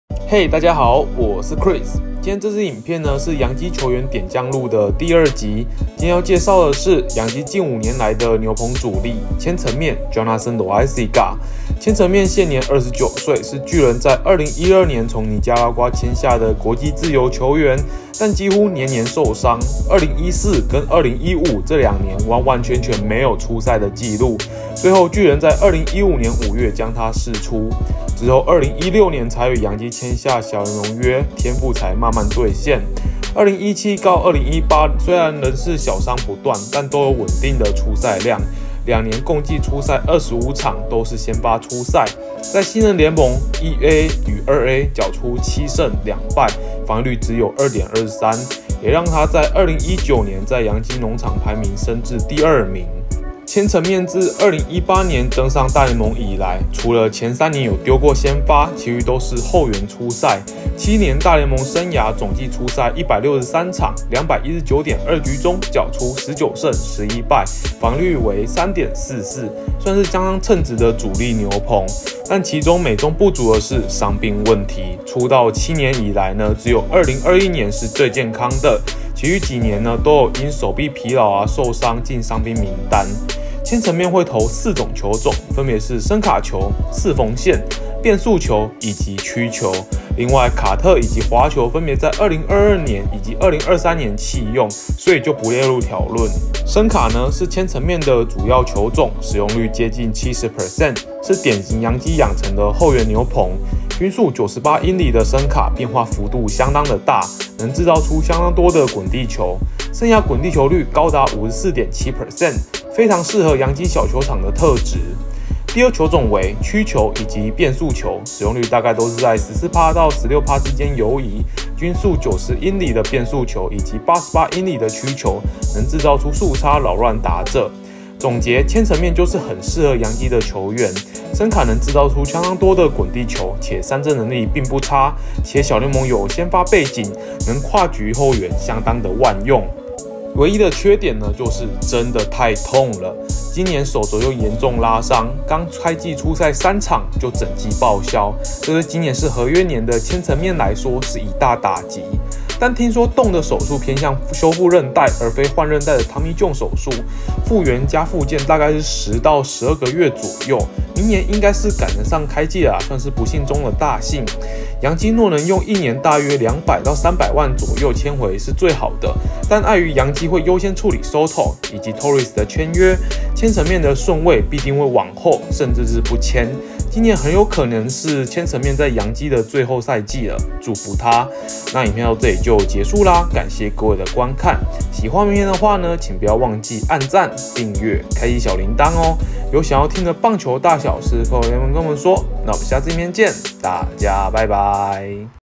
【2024洋基球員點將錄】洋基近五年主力牛棚!合約年整季報銷極度悲情!火球伸卡Jonathan Loáisiga-EP2
大家好，我是Chris。今天這隻影片是洋基球員點將錄第二集，今天要介紹的是洋基近五年來的牛棚主力，千層麵Jonathan Loáisiga。千層麵現年29歲，是巨人在2012年從尼加拉瓜簽下的國際自由球員。但幾乎年年受傷，2014跟2015這兩年完完全全沒出賽紀錄，最後巨人在2015年5月將他釋出，之後2016年才與洋基簽下小聯盟約天賦才慢慢兌現。2017到2018年雖然仍是小傷不斷，但都有穩定的出賽量，兩年總共出賽25場都是先發，在新人聯盟、1A與2A繳出7勝2敗防禦率只有2.23，也讓他在2019年在洋基農場中排名升至第2名。千層麵自2018年登上大聯盟以來，除了前三年有丟過先發，其餘都是後援出賽，七年大聯盟生涯總計出賽163場219.2局中繳出19勝11敗防禦率3.44，算是相當稱職的主力牛棚。但其中美中不足的是傷病問題，出道七年以來只有2021年是健康的，其餘幾年都有因手臂疲勞受傷進傷兵名單。千層麵會投四種球種，分別是伸卡球、四縫線、變速球跟曲球，另外卡特以及滑球分別在2022以及2023年後棄用，所以就不列入，伸卡是千層麵的主要球種使用率接近70%，是典型洋基養成後援，均速98英里的伸卡變化幅度大，能製造相當多滾地球，生涯滾地球率高達54.7%，非常適合洋基小球場特質。第二球種為曲球以及變速球，使用率都在14~16%之間游移，均速90英里的變速球以及88英里的曲球，能製造速差擾亂打者。總結千層麵就是很適合洋基的球員，伸卡能製造相當多滾地球且三振能力不差，且小聯盟有先發背景，能跨局後援相當萬用，唯一缺點就是真的太痛，今年手肘又嚴重拉傷，剛開季出賽三場就整季報銷，這對今年是合約年的千層麵來說是一大打擊，但聽說動的手術偏向修復韌帶而非換韌帶，復原加復健大約10到12個月左右，明年應該是能趕上開季，洋基若能用1年大約200~300萬左右簽回是最好的，但礙於洋基會優先處理Soto以及Torres的簽約，千層麵的順位必定往後甚至不簽，今年很有可能是千層麵在洋基的最後賽季祝福他。